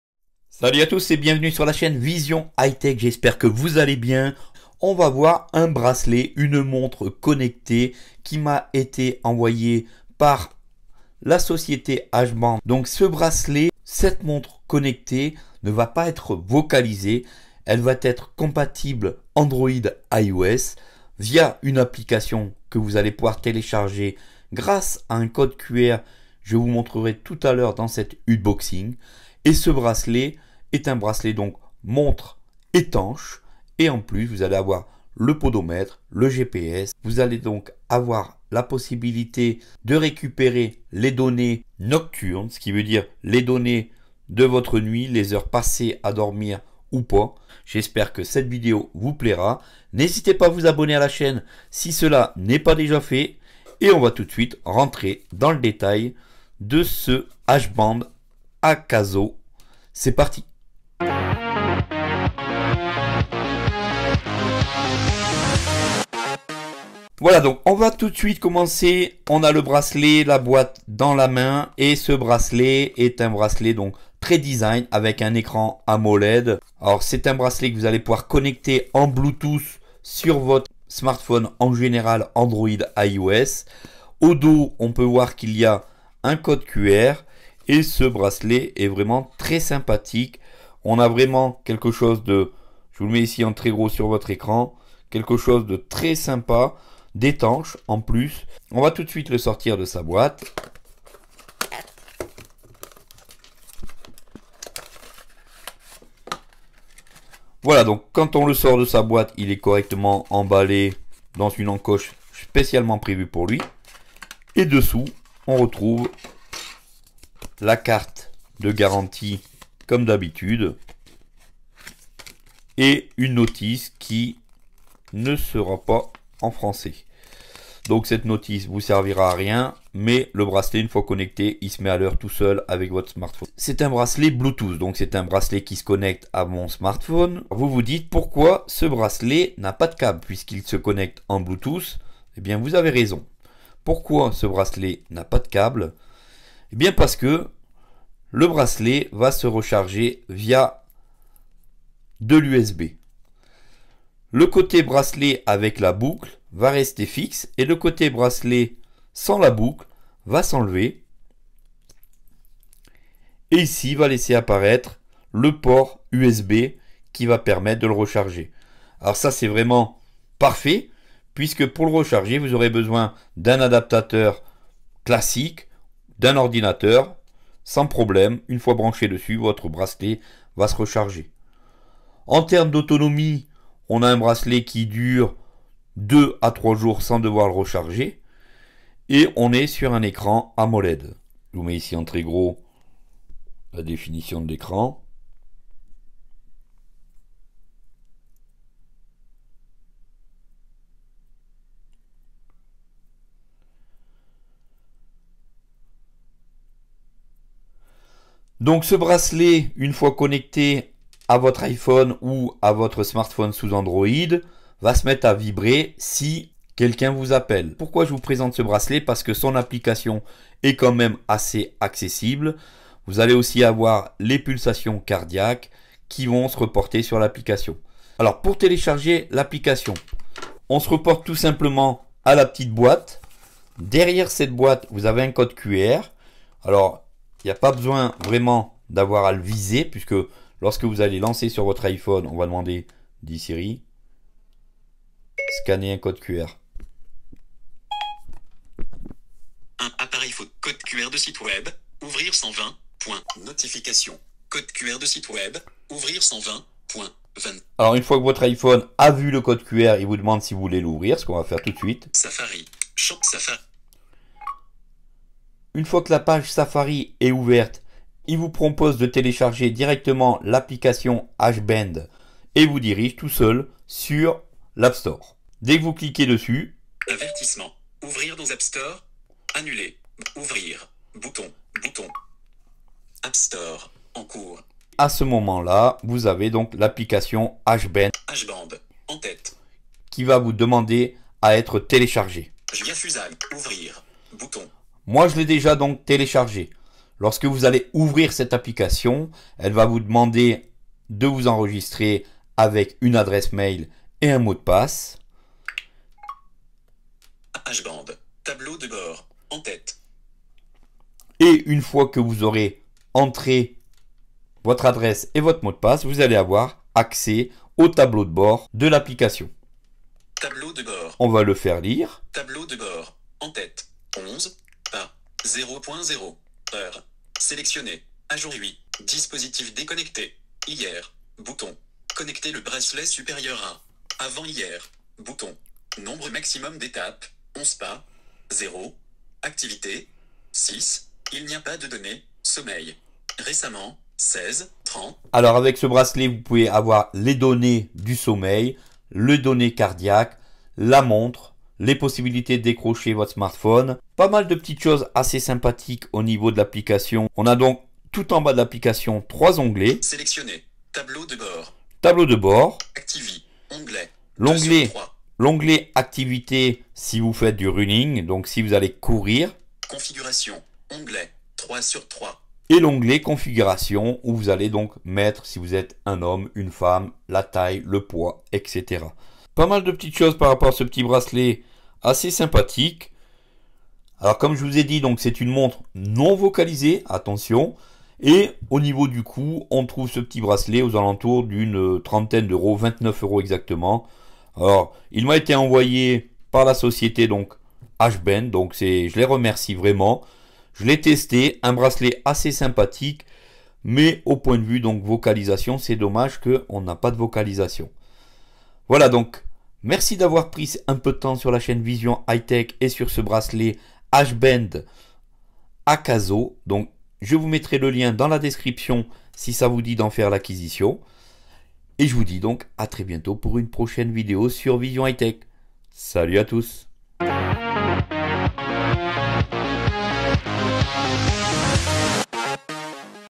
Unboxing Test Bracelet H Band AKASO Bracelet Montre cardio Podomètre Compatible iOS Android !
Salut petit bracelet connecté compatible Android, iOS H-Band AKASO, bracelet pour le Running Podomètre cardio Surveillance du sommeil. enfin Bracelet montre non vocalisé avec une application accessible. voici le lien ci dessous et je vous prépare une vidéo sur le SUNU un bracelet connecter Anti Obstacles.
lien ici: